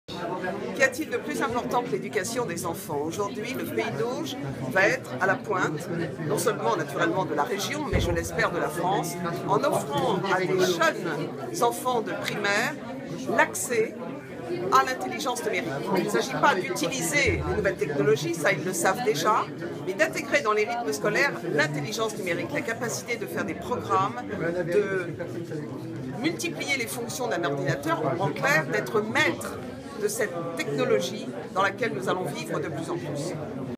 Nicole Ameline sur le projet culture code dans le Pays d'Au
Depuis la rentrée huit communes de la 4ème circonscription du Calvados proposent des cours de code numérique aux écoliers.